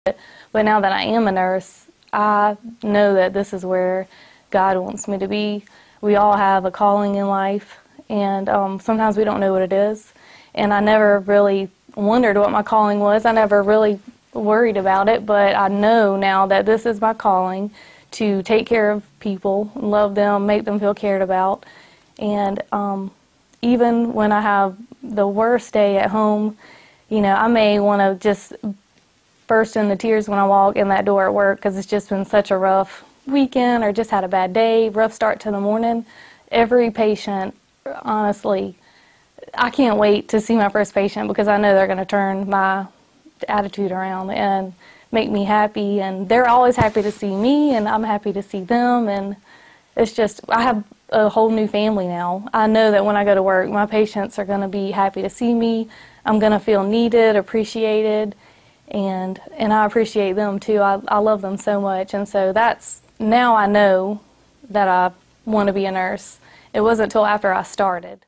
The Heart of Health Care: Lucy Strock Carter
"As a young person, I never dreamed of being a nurse. I just wanted to do something where I could help people."